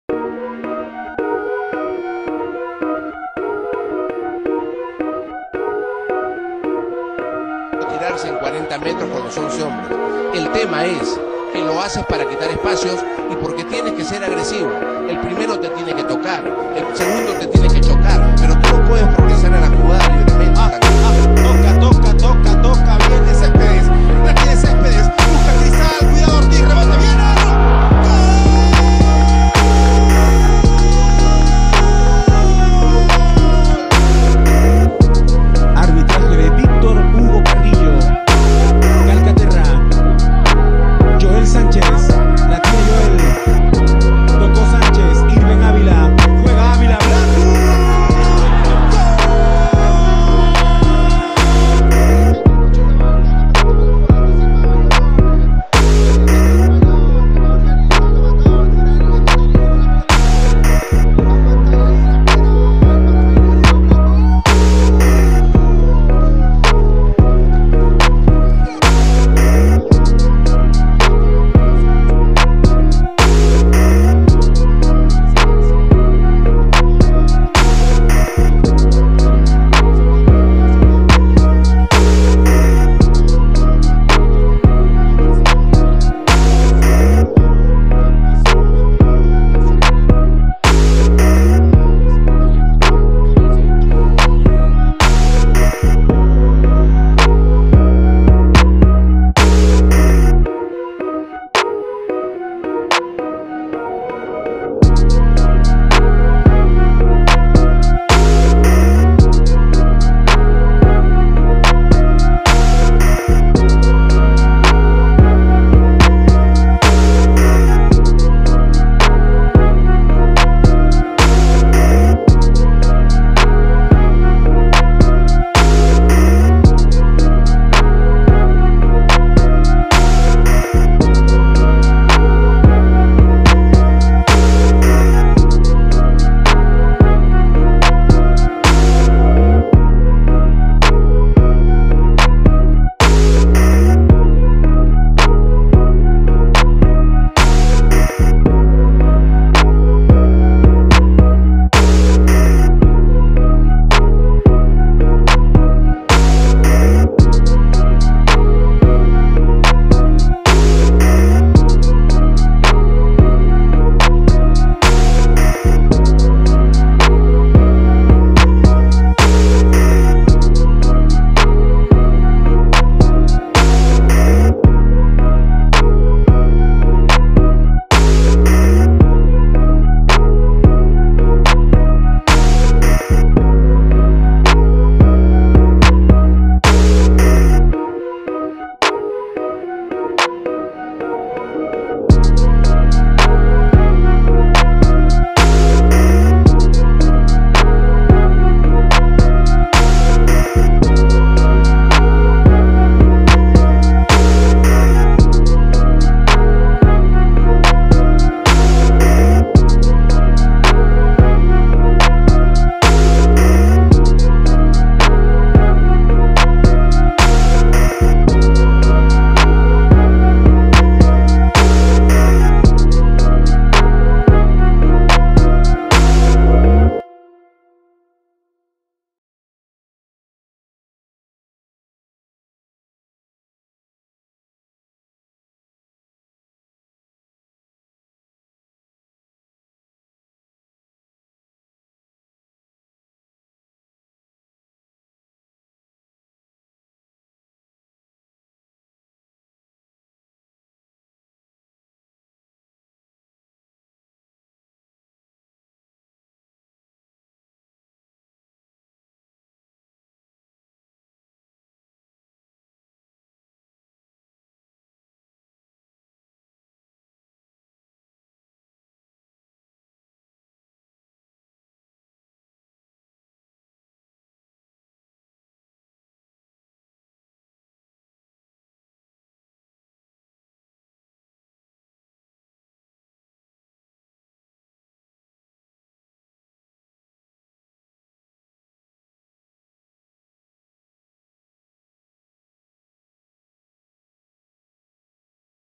Rolando Blackburn - 2017 -All Goals
Rolando Blackburn

Position:               Forward
Nationality:           Panama
Club:                     Chorrillo FC
Agent:                  Edgardo Ruben Carles Cal

This is a tall forward. He mostly plays positionally. He receives many air passes from the flanks since the player is successful in air challenges in front of opponent’s goal.